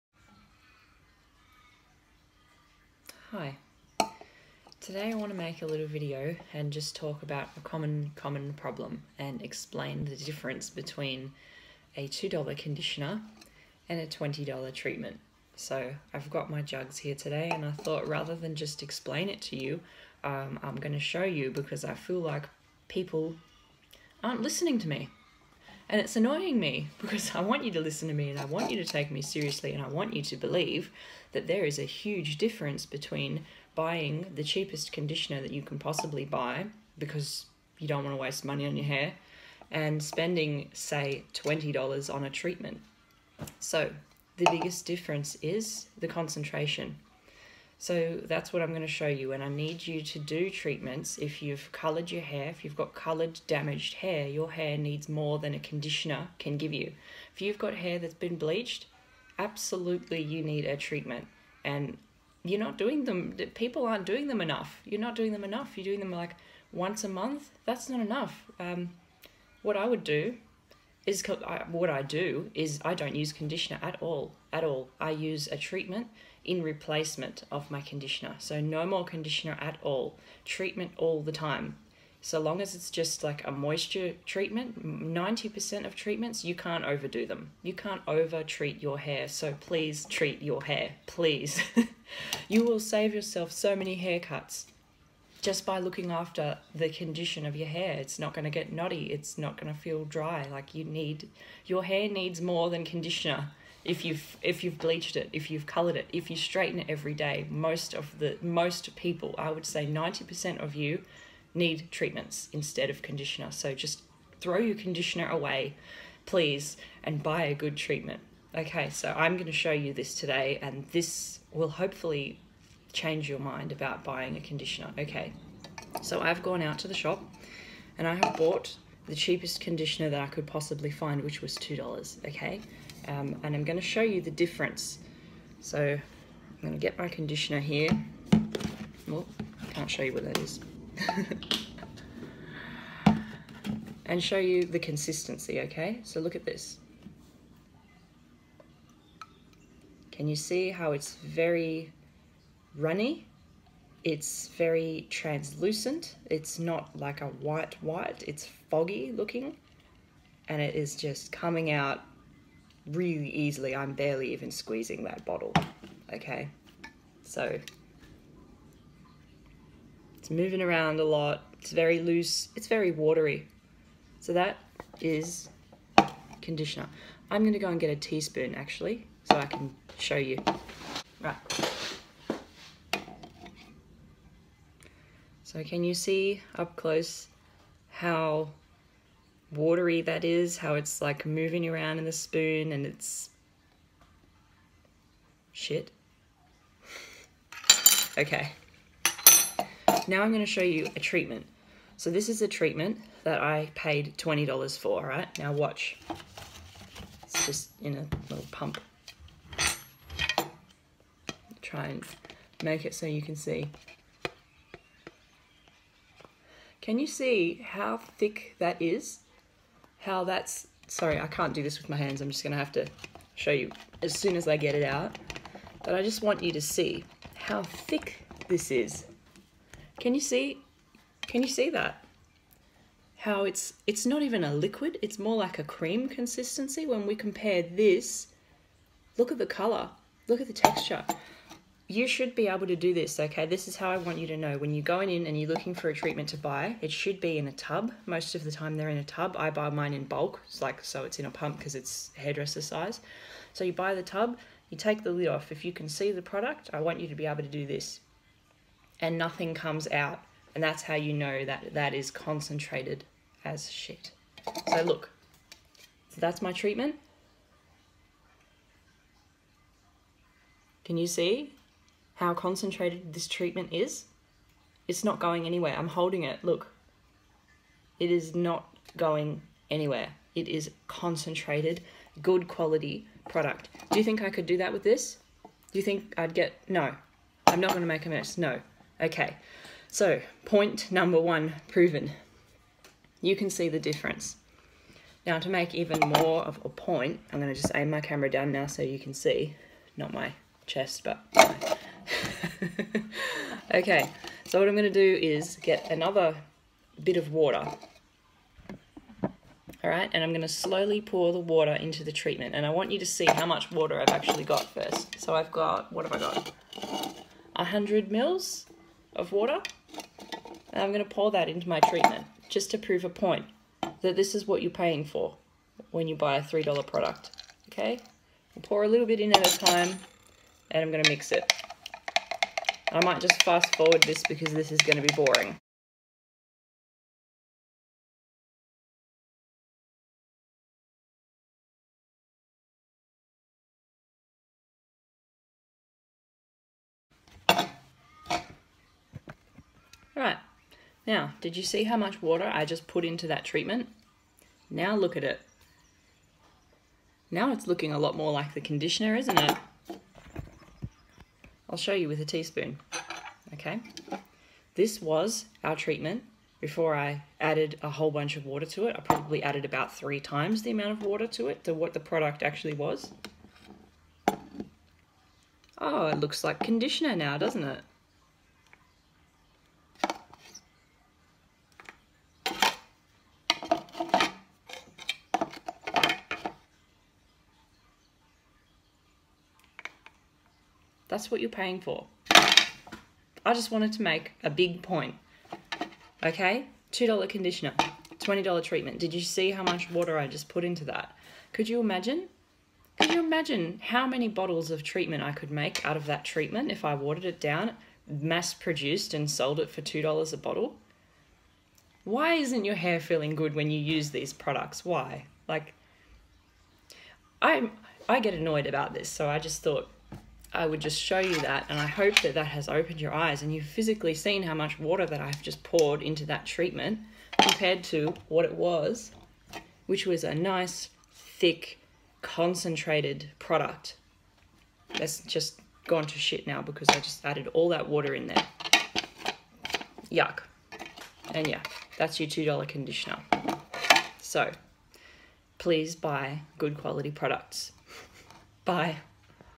CHEAP CONDITIONERS what are you really paying for??
THIS IS AN EYE OPENER!
lets talk about what you get in a $2 bottle of conditioner.
and why i want you to start doing regular treatments, and not cheap out on products!!!